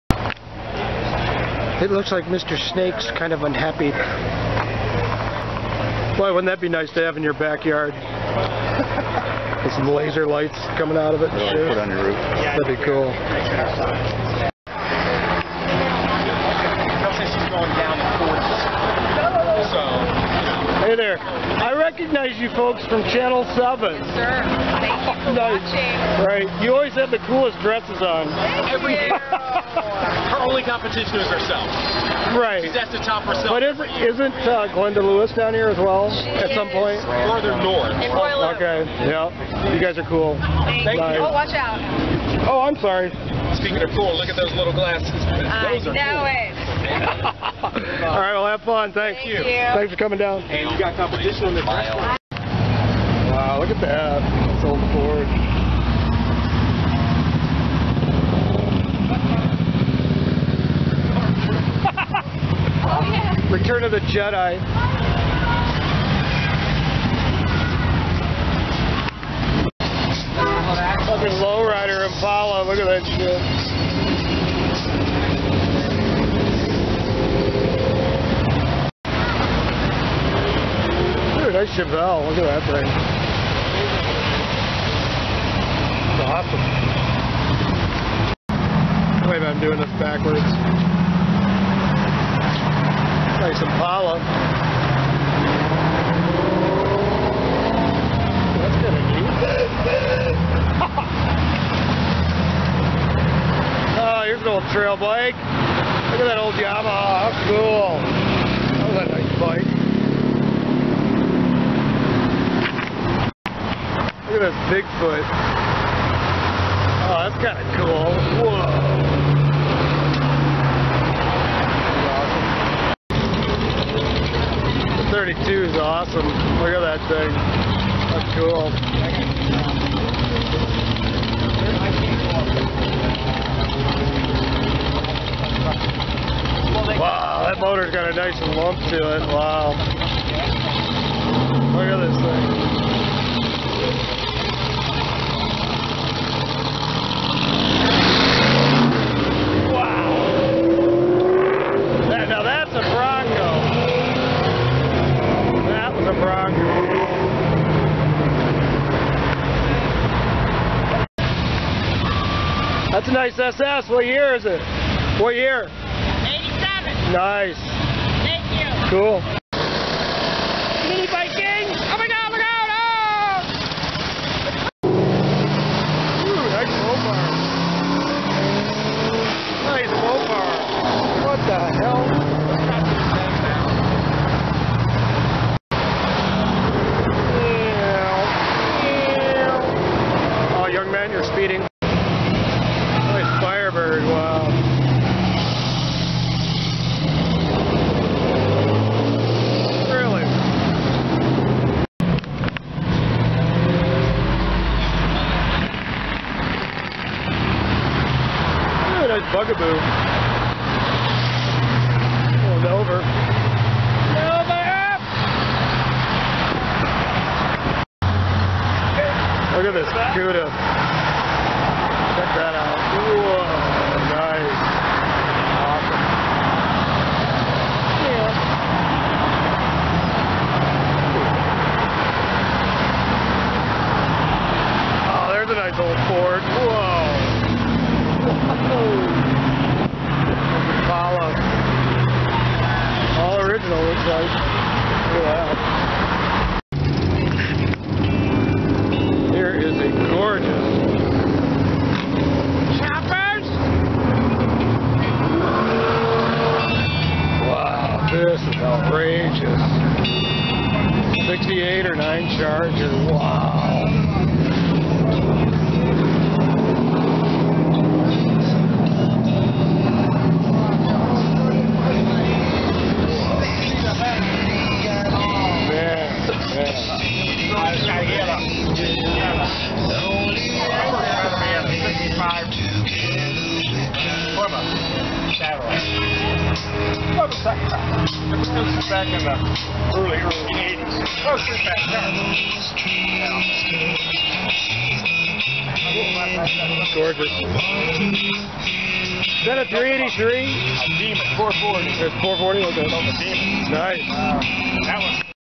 Woodwarddreamcruise 2021 #2
Snippits of the Woodwarddreamcruise 2021. No overdubbed music,,just the event and the folks and there awsome cars! Narration by a professional clown with his elf boots on.